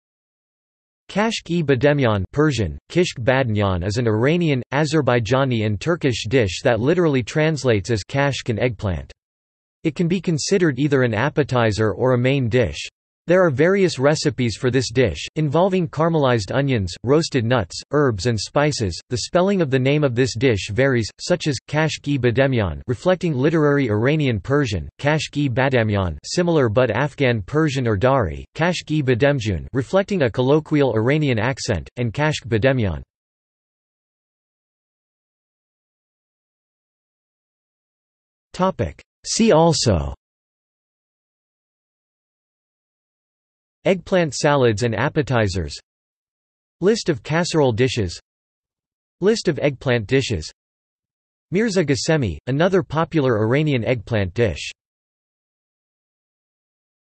Kashk e bademjan | Wikipedia audio article
This is an audio version of the Wikipedia Article:
Kashk e bademjan

00:00:34 undefined

- Socrates

SUMMARY
Kashk e bademjan (Persian: کشک بادنجان) is an Iranian, Azerbaijani and Turkish dish that literally translates as “kashk and eggplant”. It can be considered either an appetizer or a main dish. There are various recipes for this dish, involving caramelized onions, roasted nuts, herbs and spices.The spelling of the name of this dish varies, such as: kashk-e bademjan (reflecting literary Iranian Persian), kashk e badamjan (similar but Afghan Persian or Dari), kashk-e bademjoon (reflecting a colloquial Iranian accent), and kashk bademjan.